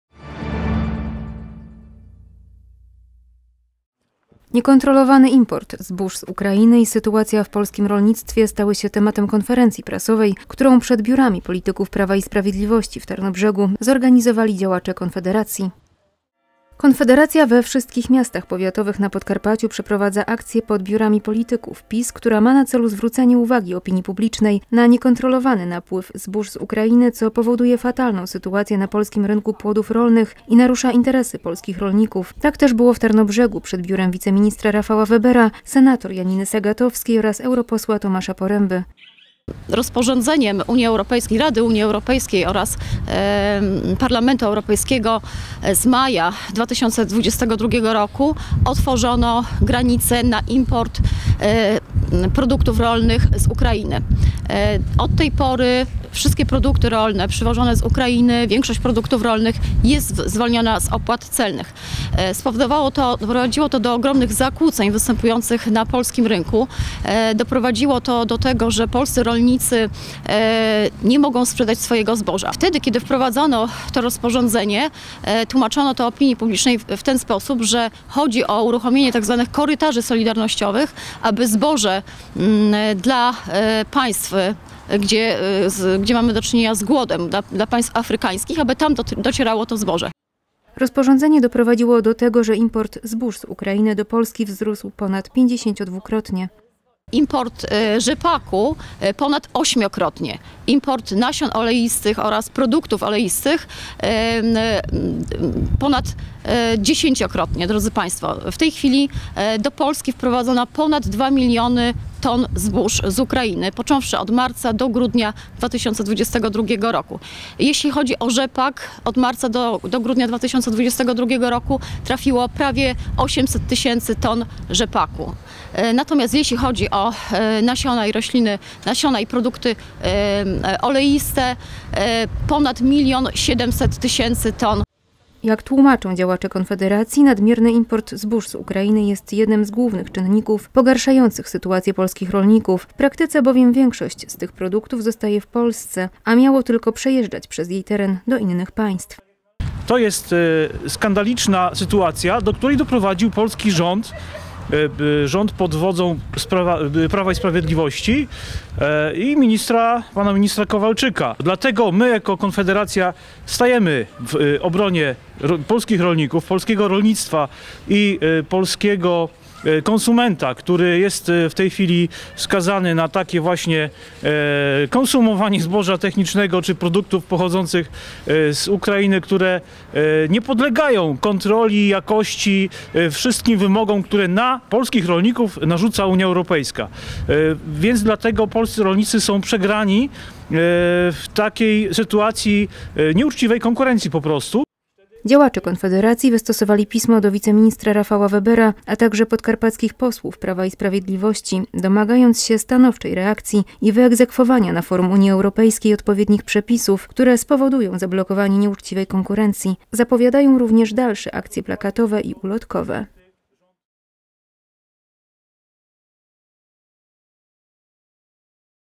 2023.02.14 - W OBRONIE ROLNIKÓW
Niekontrolowany import zbóż z Ukrainy i sytuacja w polskim rolnictwie stały się tematem konferencji prasowej, którą przed biurami polityków PiS w Tarnobrzegu zorganizowali działacze Konfederacji.